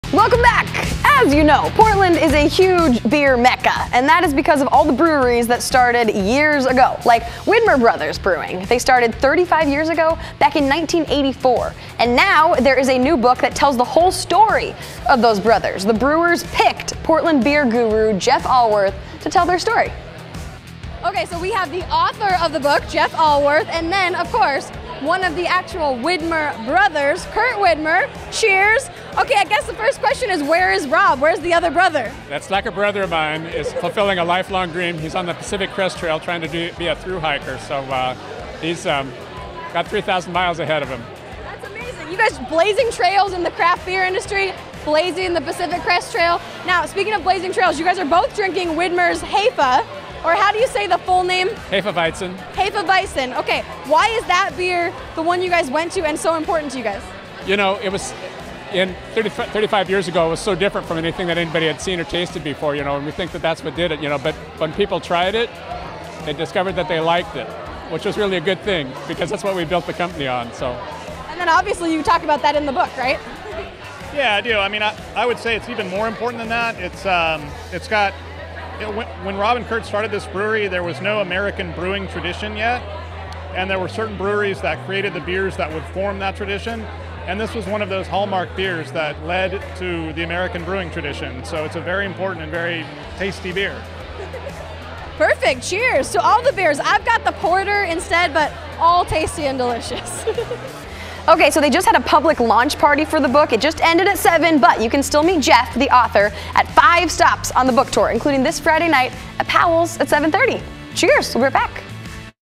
The Widmer Way book chronicles the Widmer Brothers' story
The author of a new book celebrates the history of the Widmer Brothers.